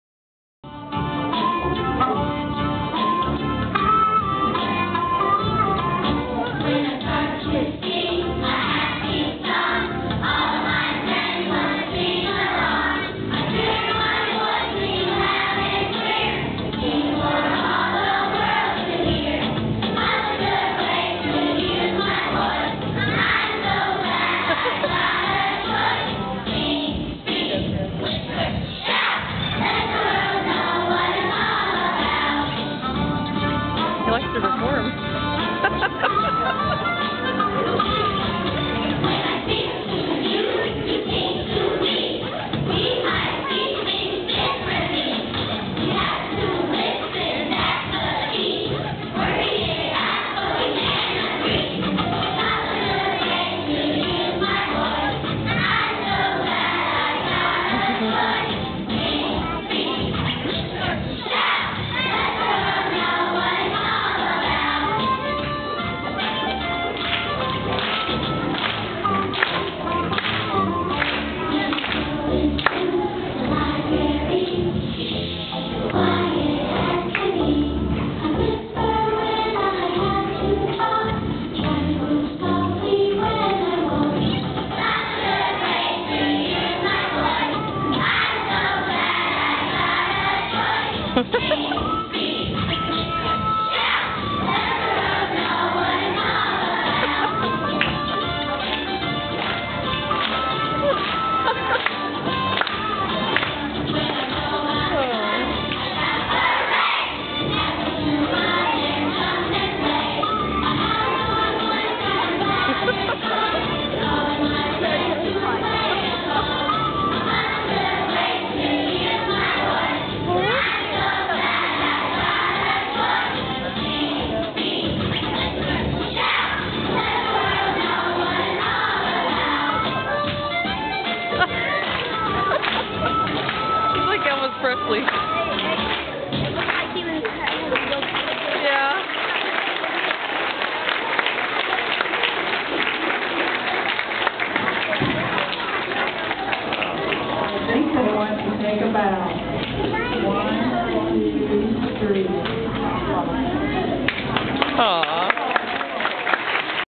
My son rocking out
I have twin sons.  The one in the front row rocking out is Jojo.  He was the life of the concert!!  My other son is also in the front row on the right side with hands in pockets the entire time.  LOL!